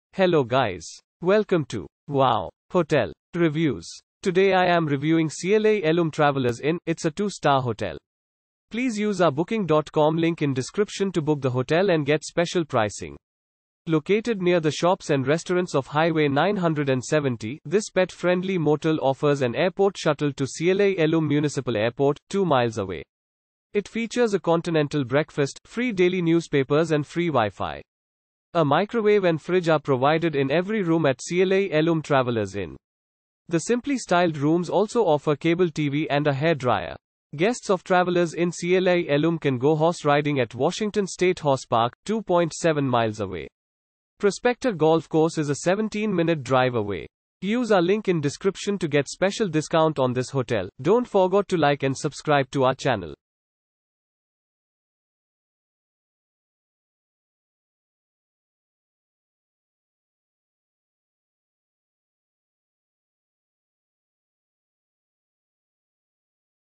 Cle Elum Travelers Inn Review - Cle Elum , United States of America
Cle Elum Travelers Inn

Located near the shops and restaurants of Highway 970, this pet-friendly motel offers an airport shuttle to Cle Elum Municipal Airport, 2 miles away. It features a continental breakfast, free daily newspapers and free Wi-Fi.
A microwave and fridge are provided in every room at Cle Elum Travelers Inn. The simply styled rooms also offer cable TV and a hairdryer.
Guests of Travelers Inn Cle Elum can go horse riding at Washington State Horse Park, 2.7 miles away. Prospector Golf Course is a 17-minute drive away.